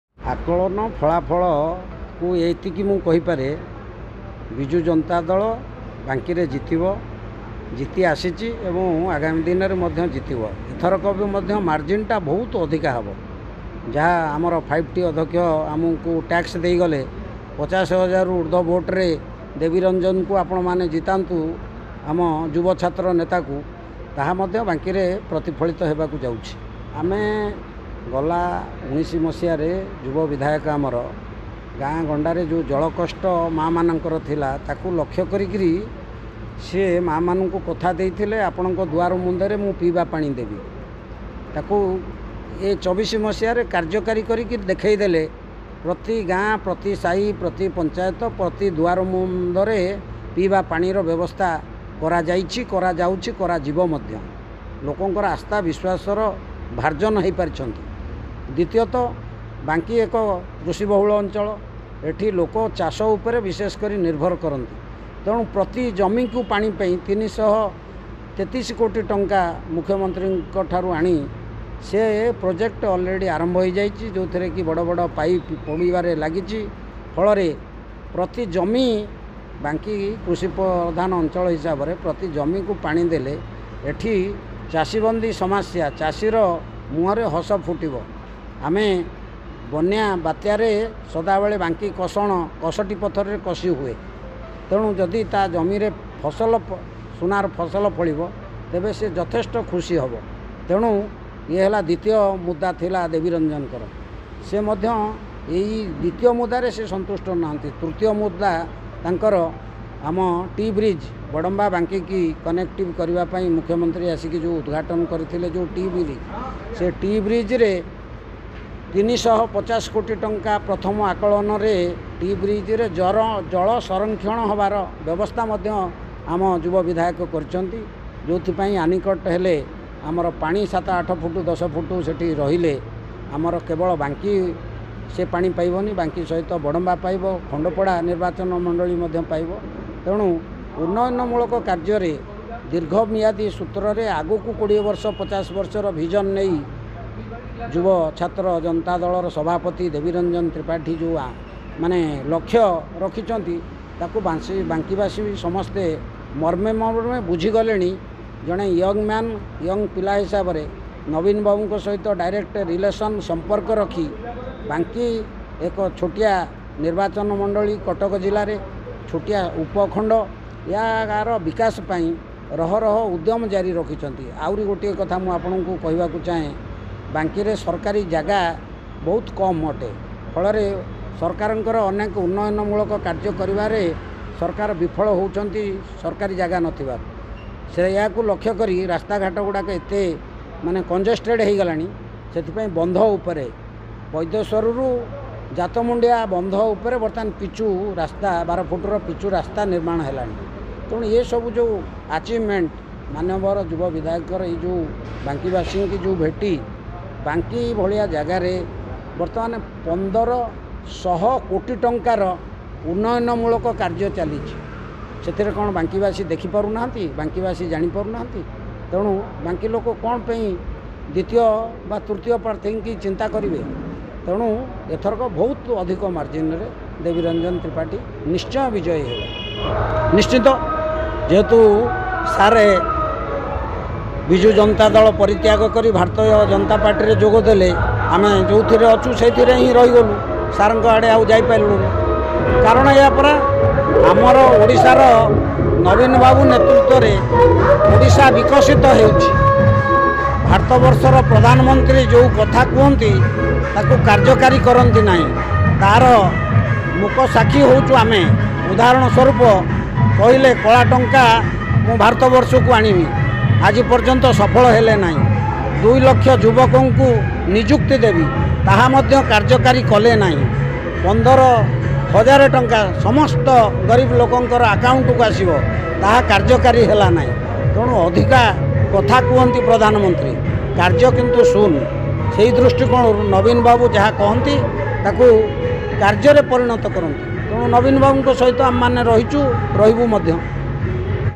MLA DEVI RANJAN TRIPATHY HAS CONTINUED HIS EFFORTS TO DEVELOP THE BANKI || BANKI POLITICS || PRIMETV
MLA DEVI RANJAN TRIPATHY HAS CONTINUED HIS EFFORTS TO DEVELOP THE BANKI || ବିଧାୟକ ଦେବୀ ରଞ୍ଜନ ତ୍ରିପାଠୀ ବାଙ୍କୀର ବିକାଶ ପାଇଁ ଅହରହ ଉଦ୍ୟମ ଜାରି ରଖିଛନ୍ତି..